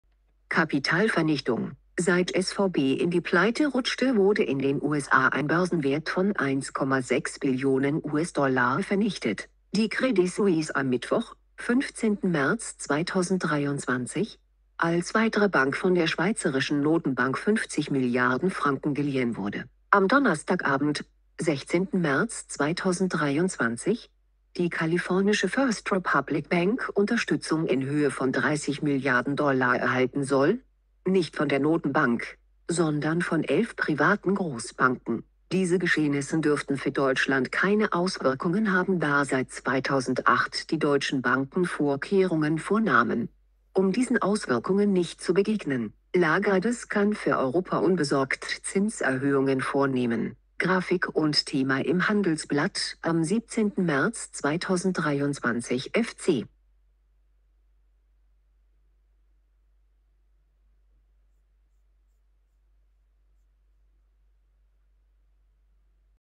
Kapitalvernichtung - Bankenkrise zu erst SVB, dann Credit Suisse nun eine weitere Bank
Kapitalvernichtung

Seit SVB in die Pleite rutschte wurde in den USA ein Börsenwert von 1,6 Billionen US-Dollar vernichtet. Die Credit Suisse am Mittwoch, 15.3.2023, als weitere Bank von der Schweizerischen Notenbank 50 Milliarden Franken geliehen wurde. Am Donnerstagabend, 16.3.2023, die kalifornische First Republic Bank Unterstützung in Höhe von 30 Milliarden Dollar erhalten soll, nicht von der Notenbank, sondern von elf privaten Großbanken. Diese Geschehnissen dürften für Deutschland keine Auswirkungen haben da seit 2008 die deutschen Banken Vorkehrungen vornahmen, um diesen Auswirkungen nicht zu begegnen. Lagardes kann für Europa unbesorgt Zinserhöhungen vornehmen. Grafik und Thema im Handelsblatt am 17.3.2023/fc